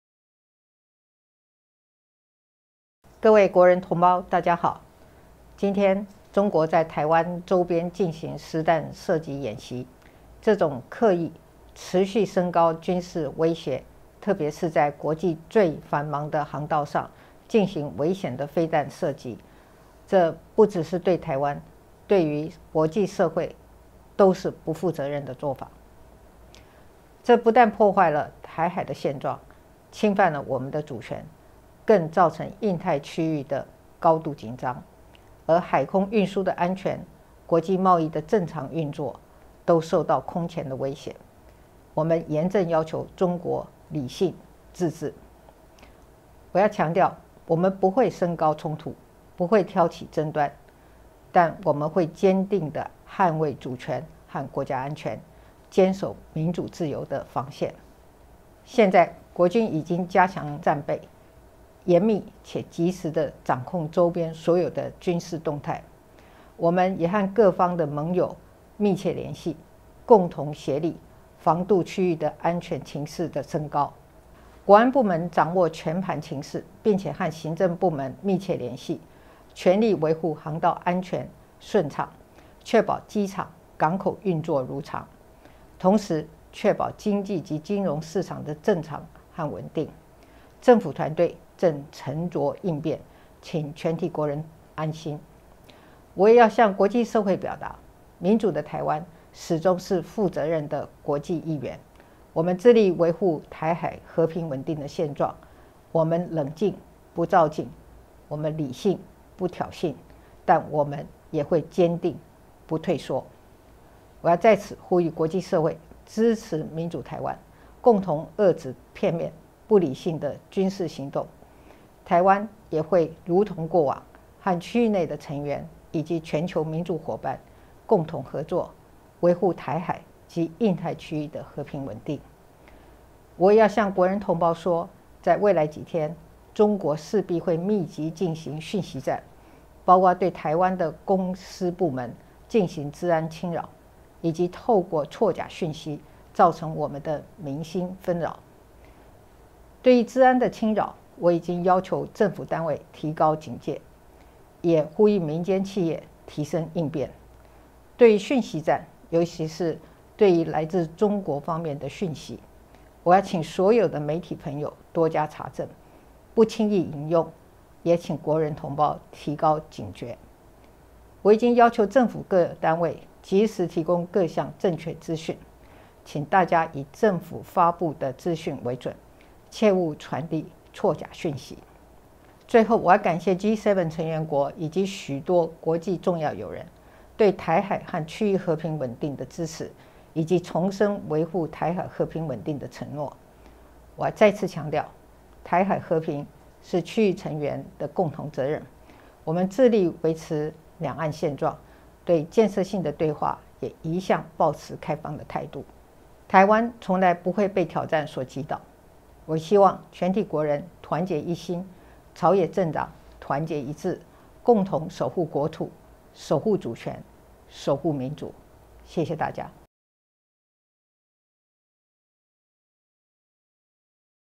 President Tsai responds to the live-fire military exercises China has initiated around Taiwan
My fellow citizens: Good evening.

Today, China initiated live-fire military exercises in areas around Taiwan. This type of continued, deliberately heightened military threat, especially the dangerous launch of missiles into some of the busiest transportation corridors in the world, is irresponsible, both to Taiwan and to the entire international community.

This not only undermines the status quo in the Taiwan Strait and encroaches upon our nation's sovereignty. It also creates high tensions in the Indo-Pacific region, as part of which air and maritime transportation safety, as well as the normal functioning of international trade, are all coming under unprecedented threat. We solemnly call upon China to act with reason and exercise restraint.

I want to emphasize that we will neither escalate conflict nor instigate disputes. But we will resolutely defend our nation's sovereignty and security as a bulwark of democracy and freedom.

As we speak, our nation's armed forces are already heightening their readiness, and are closely following in real time all military developments in surrounding areas. We are also in close contact with our allies, working together to halt further escalation of the regional security situation.

Our national security agencies are monitoring the situation, and are communicating closely with other government agencies in order to ensure that flight and shipping routes remain safe and open, that our airports and seaports continue to function normally, and that our economy and financial markets remain stable. I want to ask my fellow citizens to rest assured that our government is responding with a steady hand.

I also want to say to the international community that democratic Taiwan has consistently been a responsible member of the international community. We will work to maintain the peaceful and stable status quo in the Taiwan Strait. We are calm and will not act in haste. We are rational and will not act to provoke. But we will absolutely not back down.

Here, I want to call upon the international community to support democratic Taiwan and come together to put a halt to these unilateral and irrational military actions. As we have in the past, Taiwan will work with those in the region and democratic partners around the world to uphold peace and stability in the Taiwan Strait and the Indo-Pacific region.

I also want to say to my fellow citizens that, over the next few days, China most likely will conduct intensive information warfare operations. These will include cyberattacks on Taiwan’s public and private sectors, as well as attempts to cause psychological unrest among our people through disinformation campaigns.

To address cyberattacks, I have instructed government agencies to be on heightened alert, and I call on private enterprises to step up their response measures. To address information warfare, especially information from Chinese sources, I ask all our friends from the media to be even more meticulous when fact checking and extra careful with attribution. I also ask my fellow citizens to remain alert.

I have directed all government agencies to provide correct information as soon as possible. Please take the information from our official government sources as accurate, and remember not to share unverified information.

Lastly, I want to thank the G7 countries and our many good friends around the world for supporting peace and stability in the Taiwan Strait and the region, and for reiterating their firm commitment to maintaining peace and stability in the Taiwan Strait.

I want to emphasize again that peace in the Taiwan Strait is the shared responsibility of everyone in the region. We are doing our utmost to uphold the cross-strait status quo, and we remain open to constructive dialogue.

Taiwan does not give in to challenges, and it never will. I hope that everyone in Taiwan comes together, and that all political parties stand united to jointly defend our nation's territory, our sovereignty, and our democracy. Thank you.